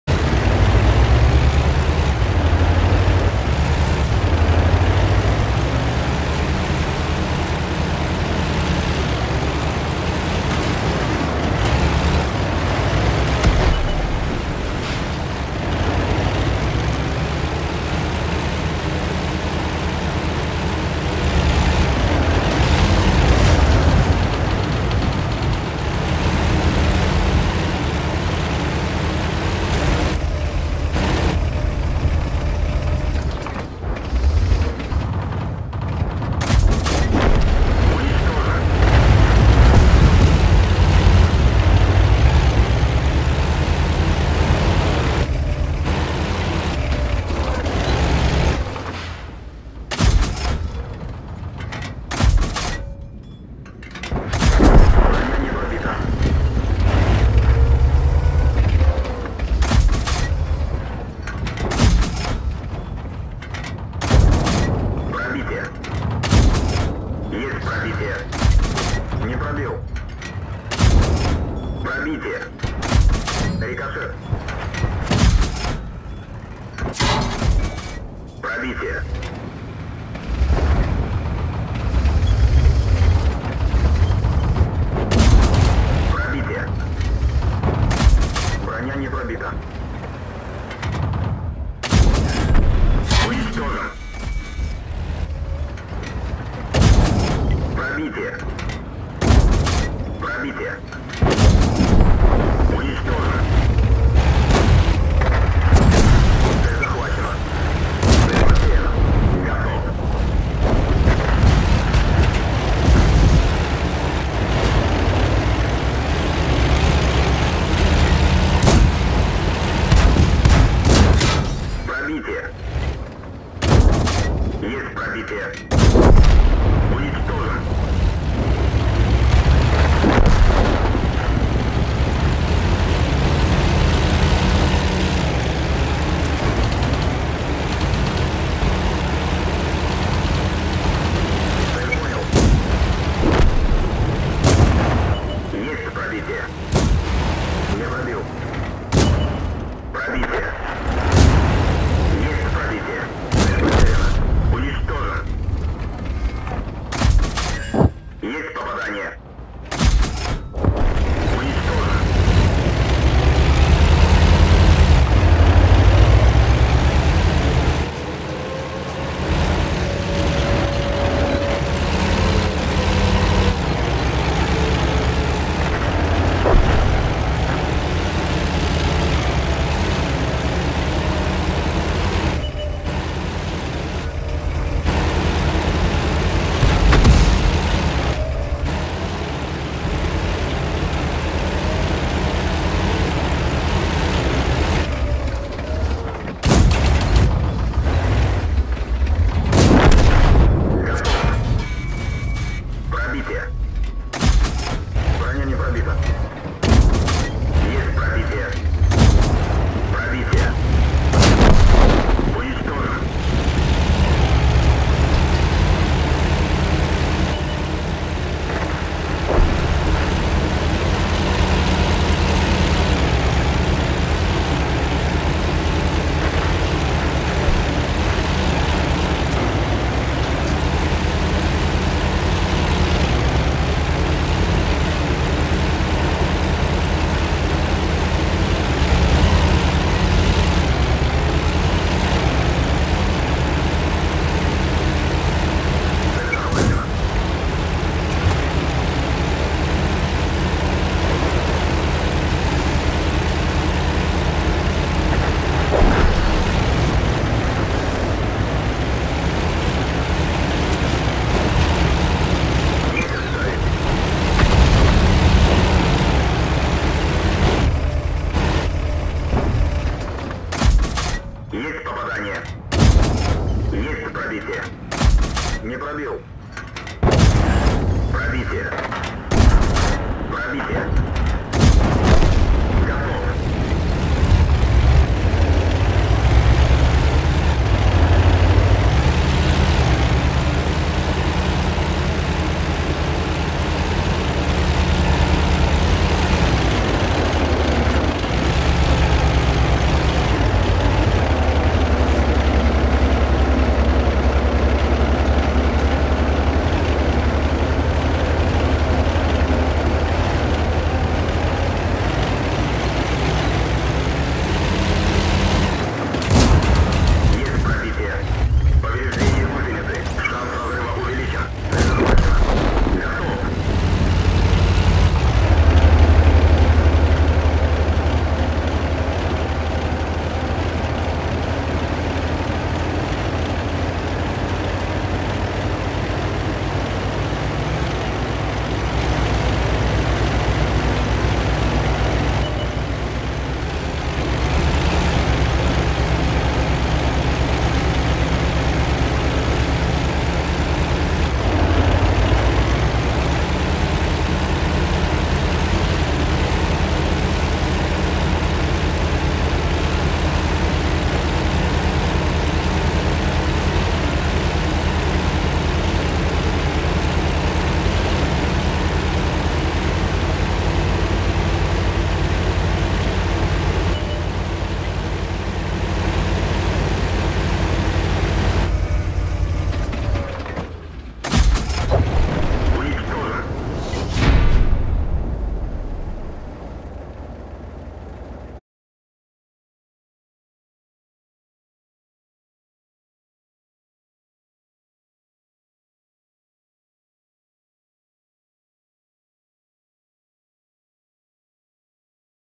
т-127 ( 12 фрагов )
просто нагибчик в песке на т-127 (12 фрагов )
на война или на разминочку пж :Р